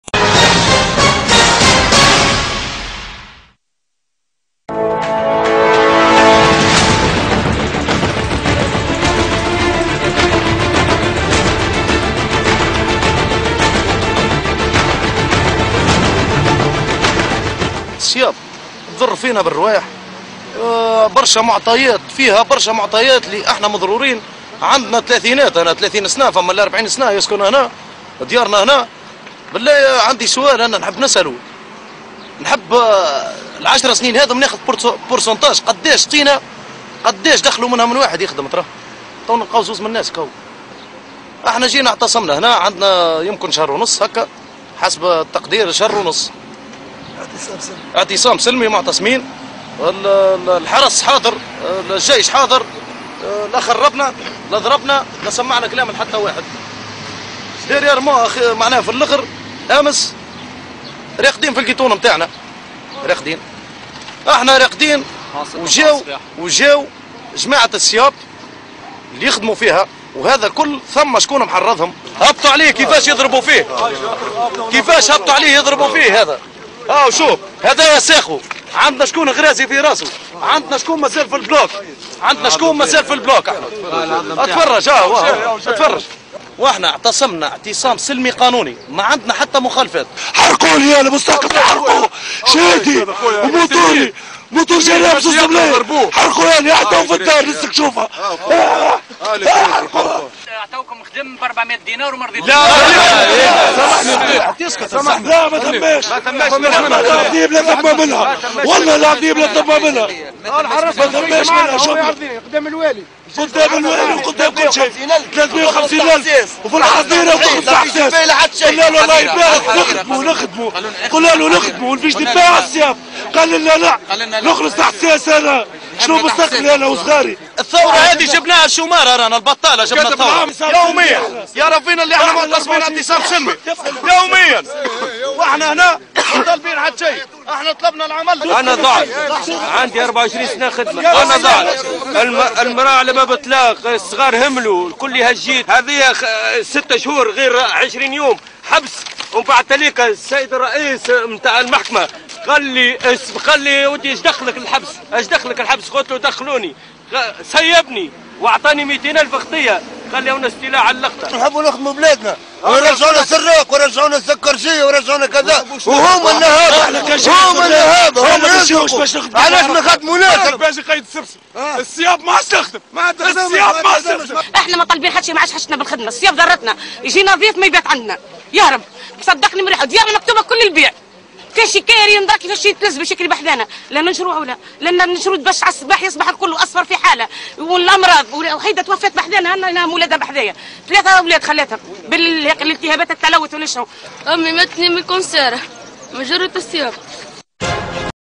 اعتصامات و مواجهات
اعتصام سلمي تحول الى مواجهات بين العاطلين عن العمل و عمال السيياب...
تشكي المتساكنين المتواجدين قرب شركة سيباب من التهابات التلوث...